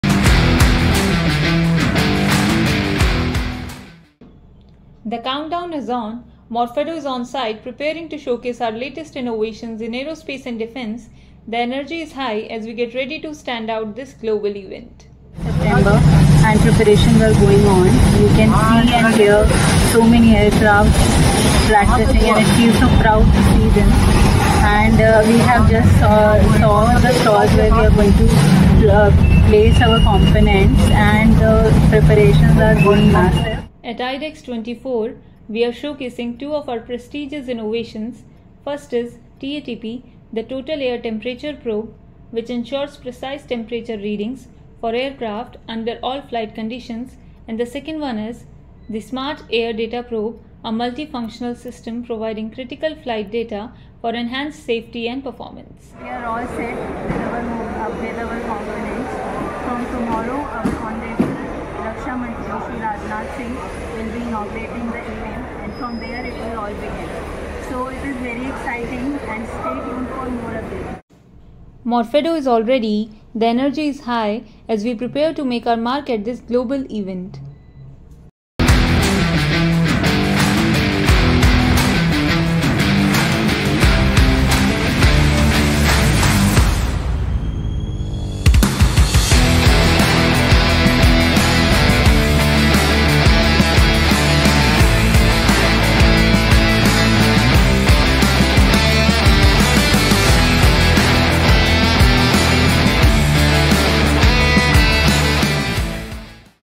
Morphedo at International Defence Aviation Exposition IDAX-24
🚀 Proud Moment! 🚀 We're thrilled to announce that Morphedo has been selected by DRDO to exhibit at International Defence Aviation Exposition IDAX-24 (TARANG SHAKTI), one of the world’s leading platforms for Aerospace & Defence innovations. 🌍✈️

We are excited to showcase two of our innovative products currently in development:

1. Total Air Temperature Probe (TATP) – providing accurate total air temperature measurements for Light Combat Aircraft (LCA), with advanced sensing technologies and robust de-icing capabilities.

2. Smart Air Data Probe – an all-in-one sensor system for LCA, consolidating critical air data like static pressure, total pressure, angle of attack, and angle of sideslip.

At Morphedo , we are dedicated to innovating for a self-reliant India, contributing to the nation’s mission of Aatmanirbhar Bharat in Aerospace and Defence.

Looking forward to networking, collaborations, and exploring new advancements in aviation technology at International Defence Aviation Exposition IDAX-24 (TARANG SHAKTI)